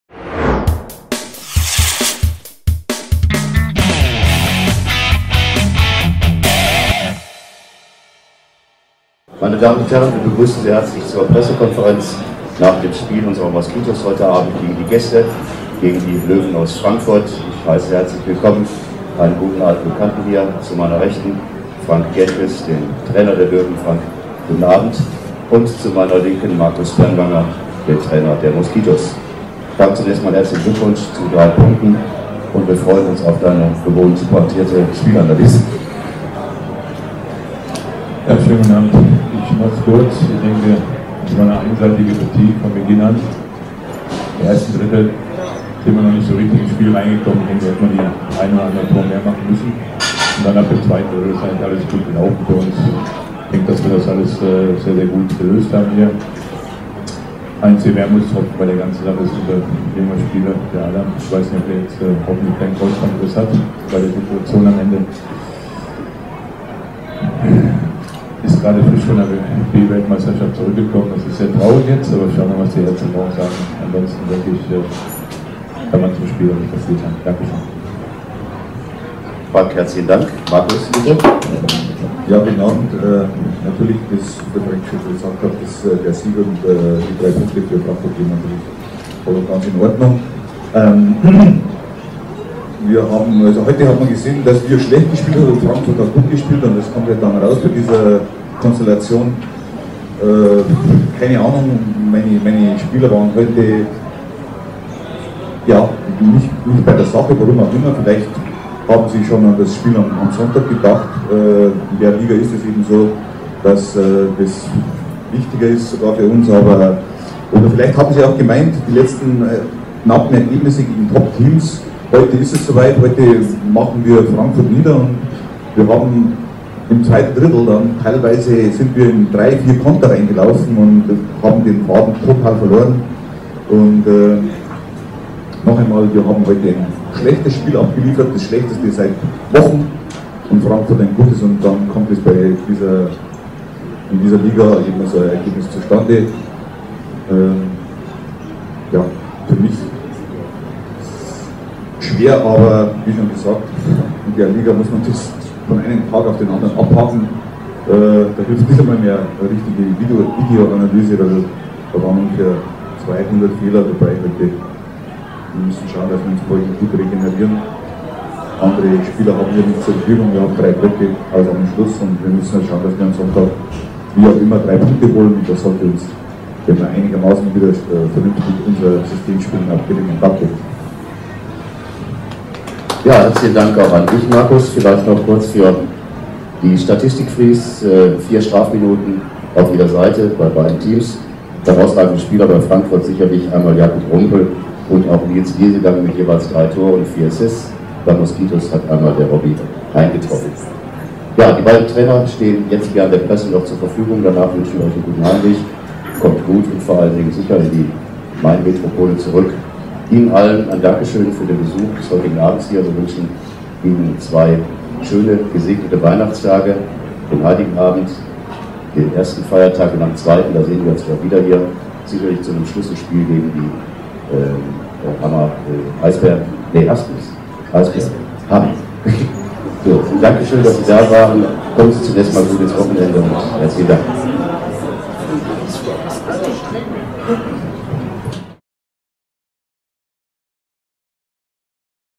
Pressekonferenz: ESC Moskitos Essen - Löwen Frankfurt (21.12.2012)
Am Ende siegen unsere Löwen mit 14:1 in Essen - und Nils Liesegang erhält als bester Spieler ein Fass Bier. Die Treffer im letzten Abschnitt gingen auf das Konto von Roland Mayr, Adam Domogalla, Patrick Schmid, Nils Liesegang und Norman Martens.